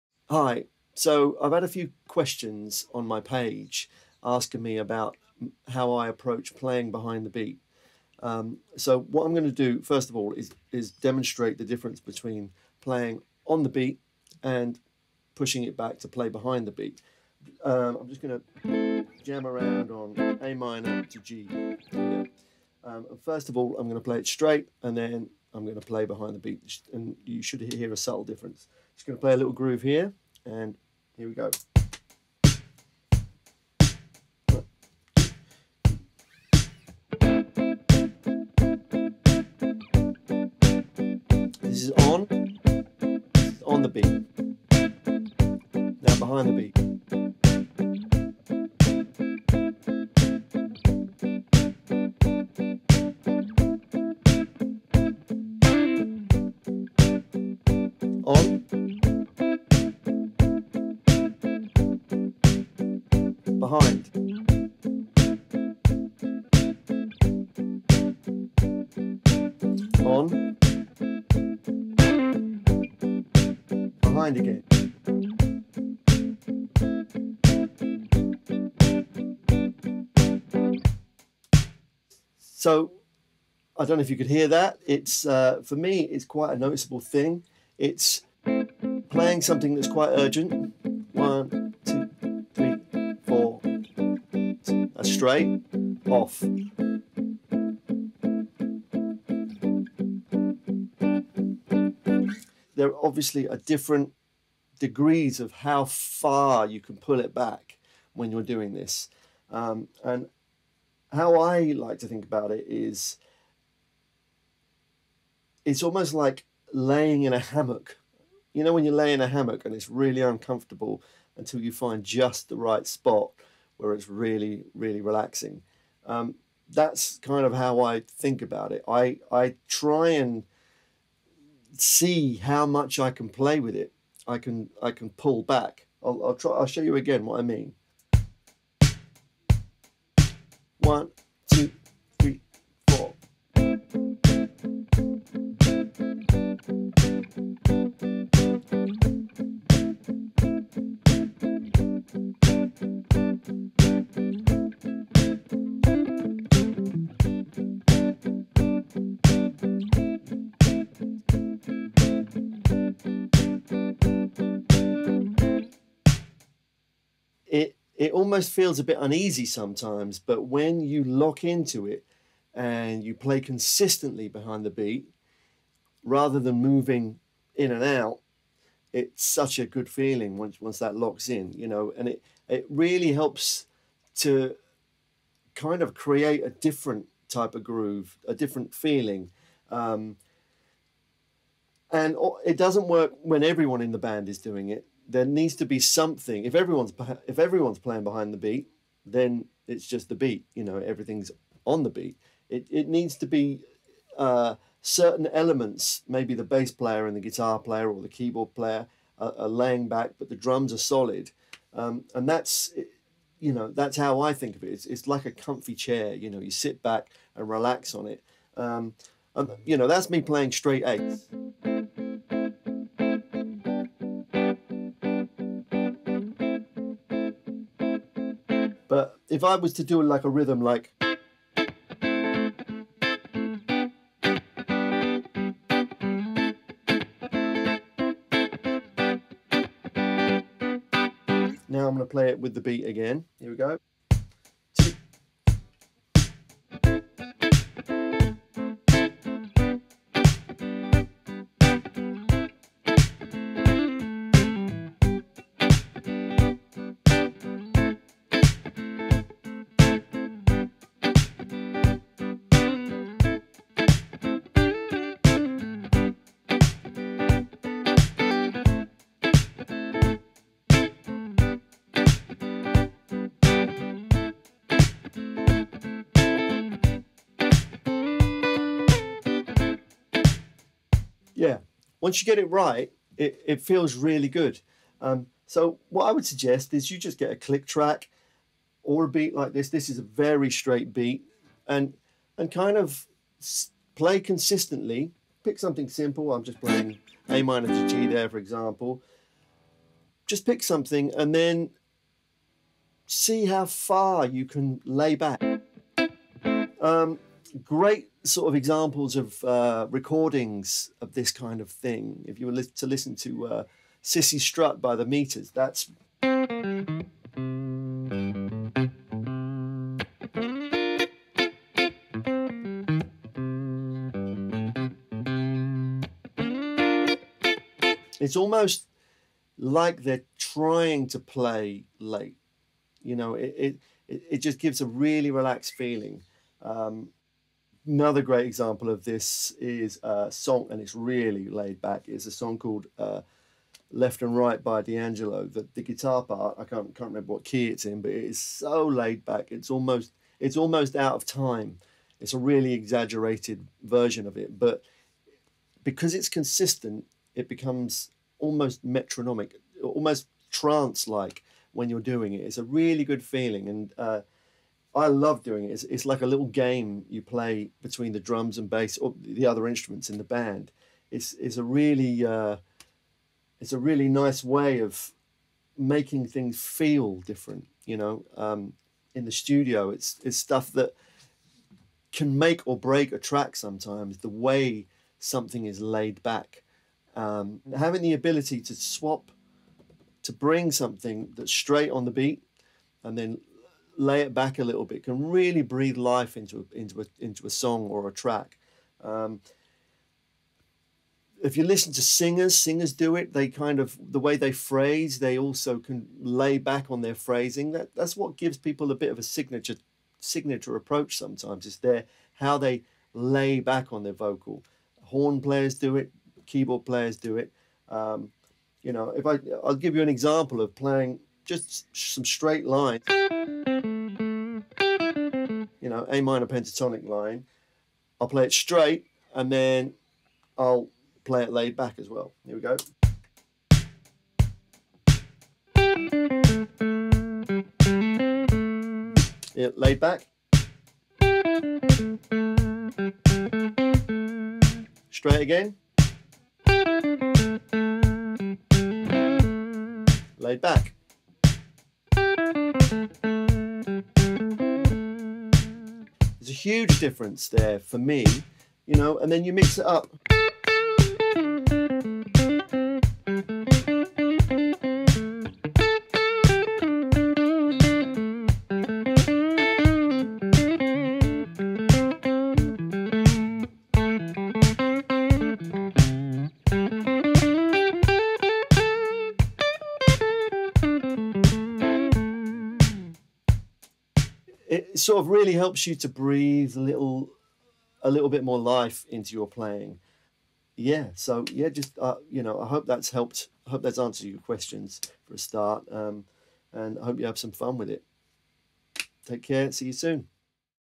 How to play play BEHIND THE BEAT!
A quick video about laying back on the beat.